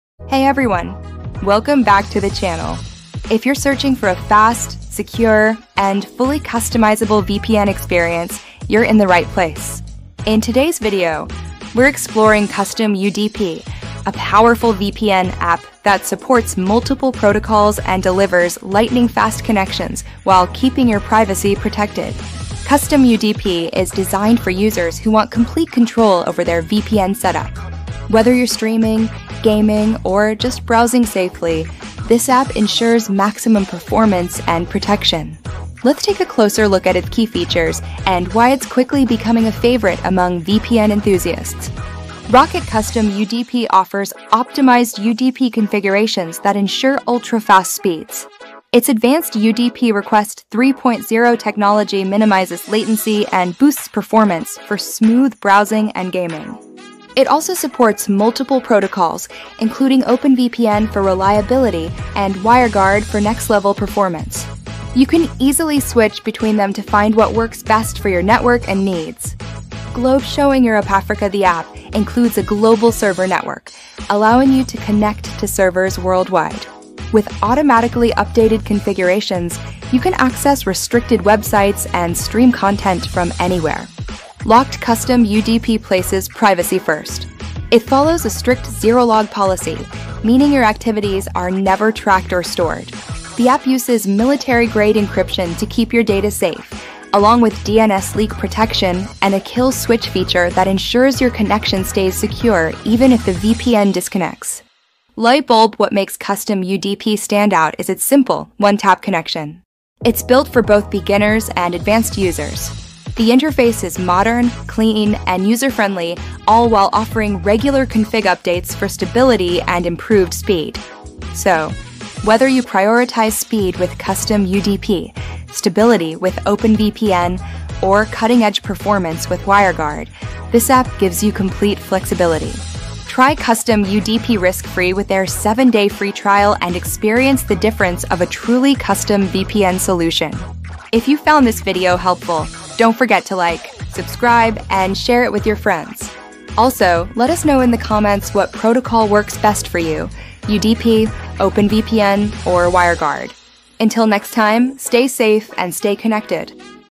Custom UDP - Fast & Secure VPN | Multiple Protocols with OpenVPN, WireGuard & UDP 3.0
Experience ultimate online speed and privacy with Custom UDP – the fast and secure VPN app that supports multiple protocols including Custom UDP, OpenVPN, and WireGuard. In this video, we’ll show you how to use Custom UDP to enhance your internet security and achieve ultra-fast connections anywhere in the world.

With advanced UDP Request 3.0 technology, Custom UDP delivers smooth, low-latency performance ideal for streaming, gaming, and browsing. Protect your privacy with a zero-log policy, military-grade encryption, and DNS leak protection – all while enjoying a simple one-tap interface. Whether you prefer the reliability of OpenVPN, the power of WireGuard, or the lightning-fast speed of Custom UDP, this VPN ensures complete freedom online.

▶️ 125tech.online